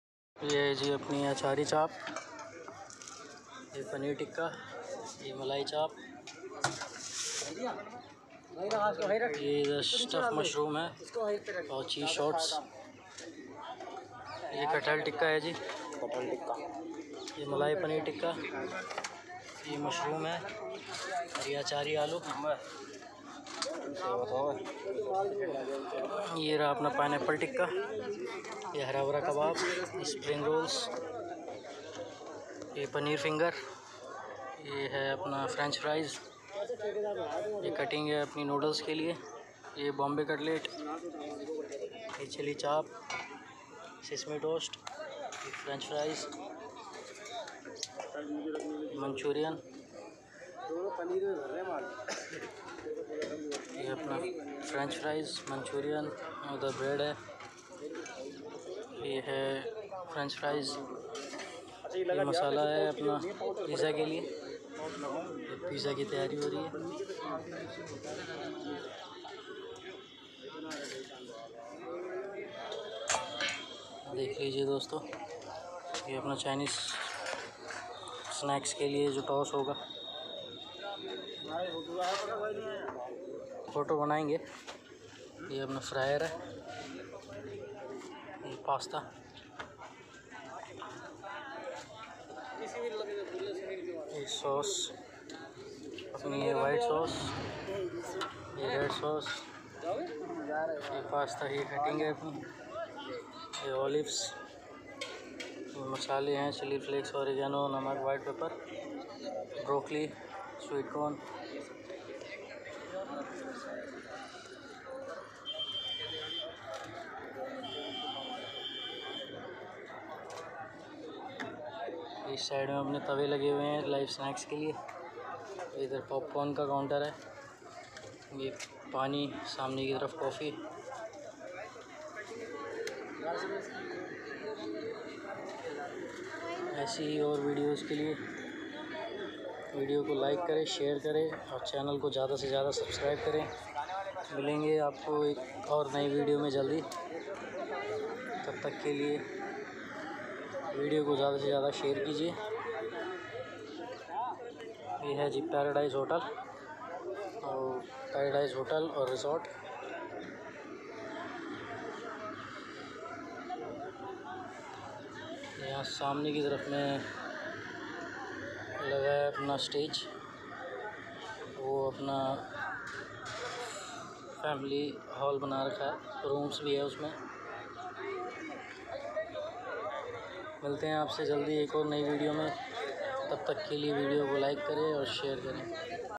Vlog 2 Paradise Resort Alwar Rajasthan 🏨
We Are in Paradise Resort For Making Chinese Snacks Foods pizza,pasta Grilled Snacks And Salads.
Like the Video And share the video For more new Videos.